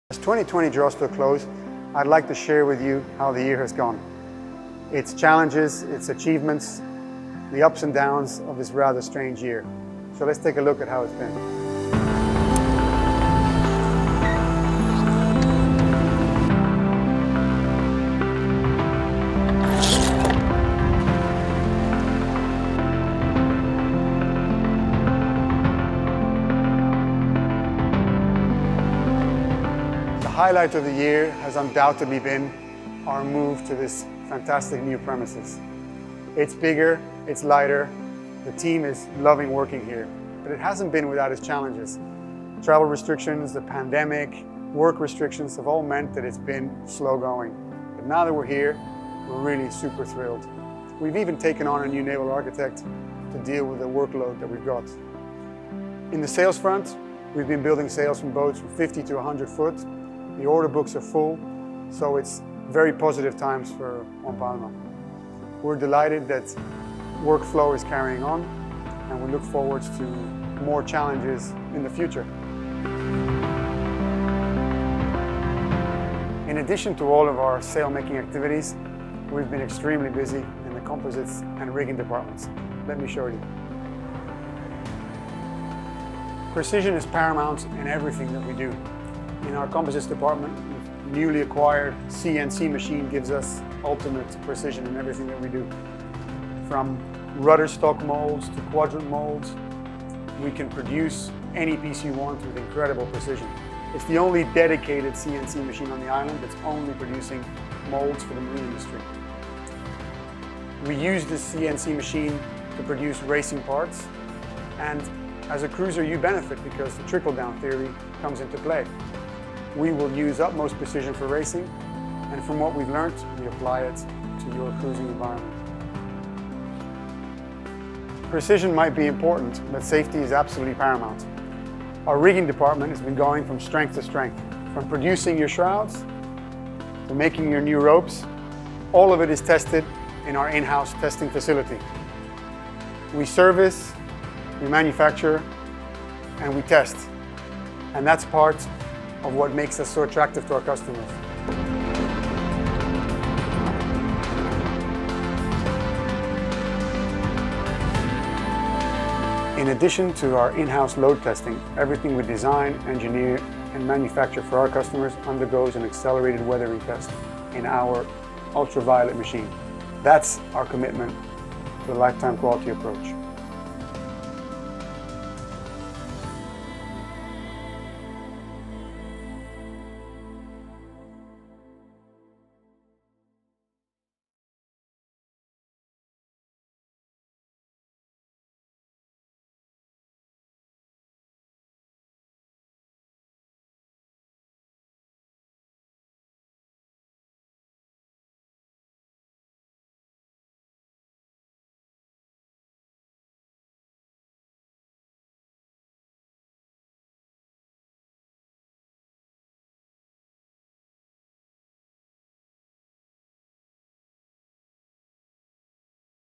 ONE Palma 2020
As 2020 draws to a close, I'd like to share with you some of the highlights of our year, to counter  the obvious global challenges we have all endured.

1. Completed the build of our all-new production facility, despite strict lockdown measures: bigger, brighter and significantly  more energy efficient.
2. Added a second Naval Architect to the technical department.
3. Increased sails production for 60-100ft yachts.
4. Acquired a 3-axis CNC machine, solely dedicated to manufacturing high precision molds and composite components for the marine industry.
5. Invested in specialist rigging equipment for the manufacture of stainless steel standing rigging.
6. Completed our in-house testing facility with Destructive, non-destructive and accelerated-weathering UV testing equipment.